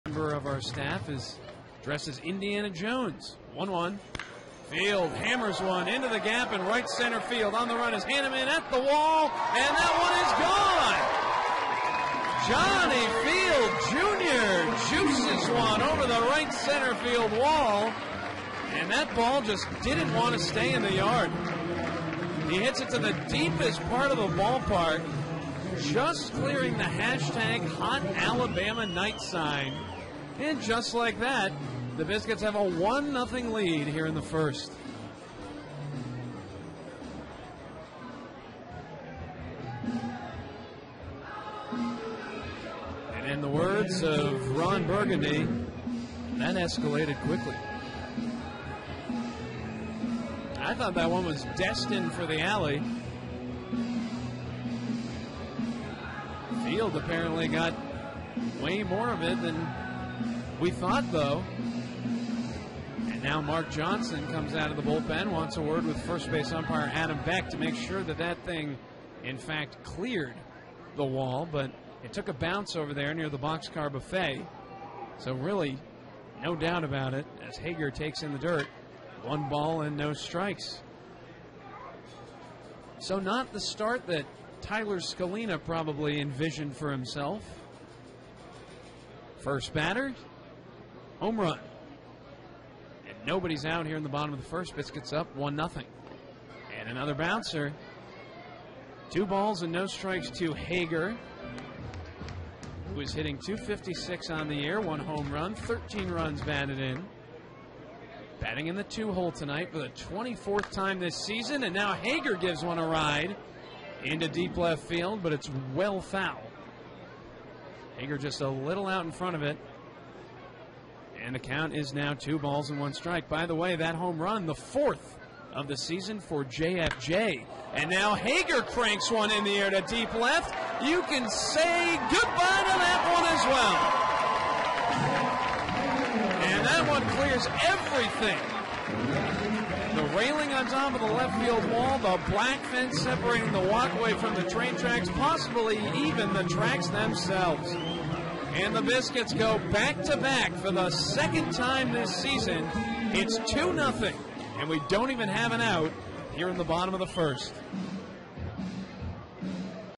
Johnny Field and Jake Hager go back-to-back
5/12/16: Johnny Field and Jake Hager hit back-to-back jacks to lead off the bottom of the first inning against Tennessee